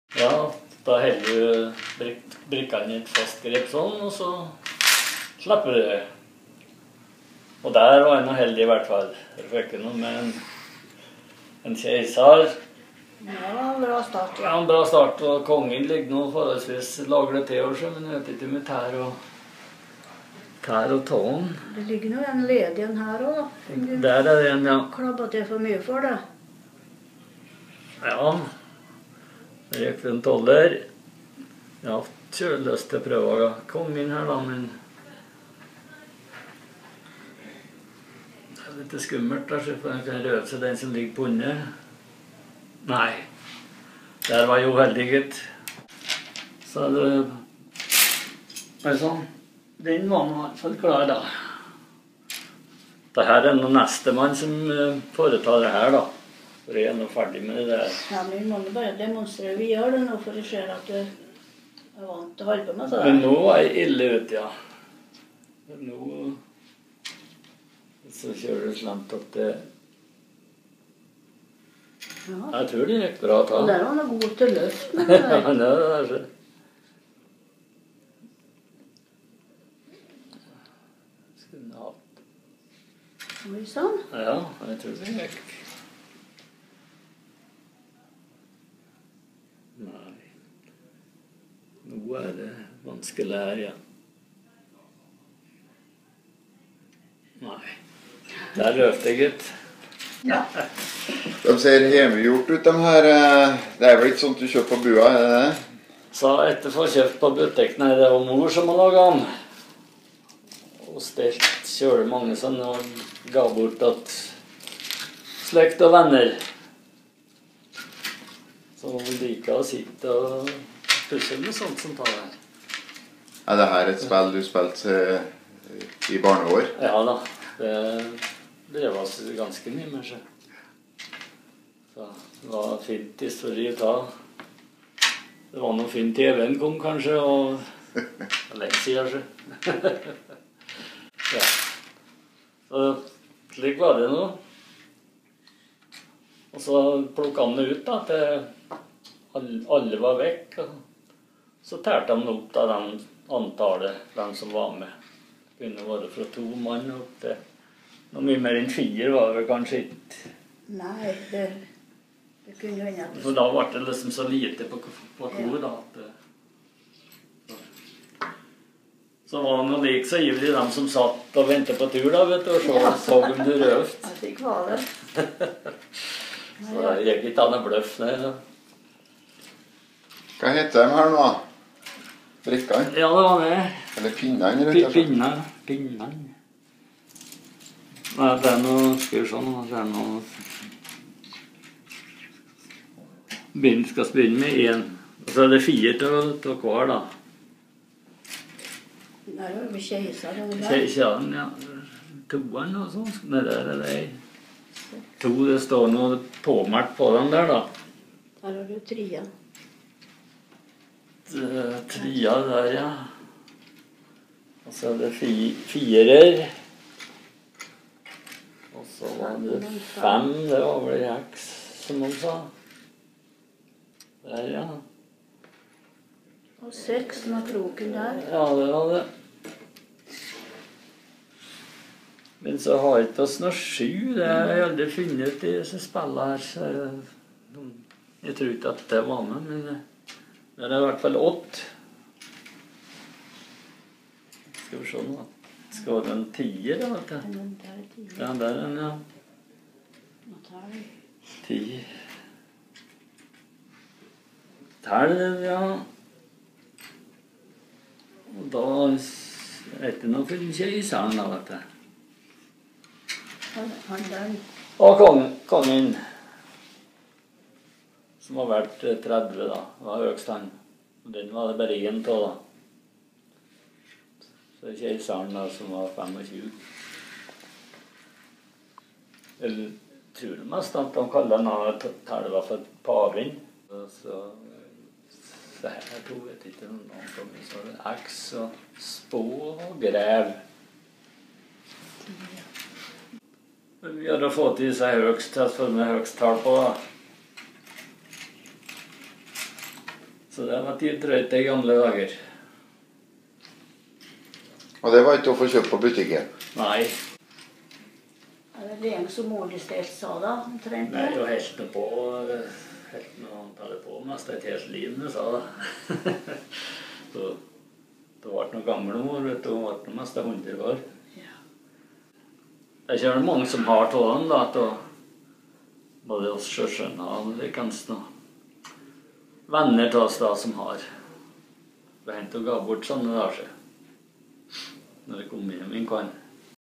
Pinnespill i Oppdal
Georg Slipersæter og Pauline Ishoel spiller lokalt spill fra Oppdal.